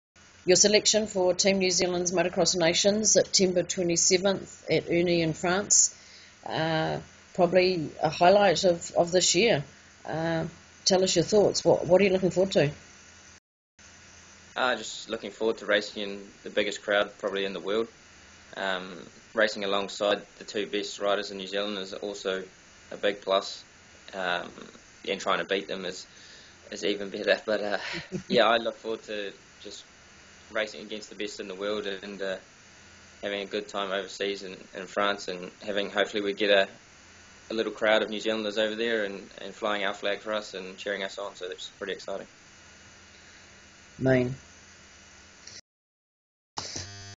Kayne Lamont MXON NZ Team Selection
Sit down and talk with Kayne Lamont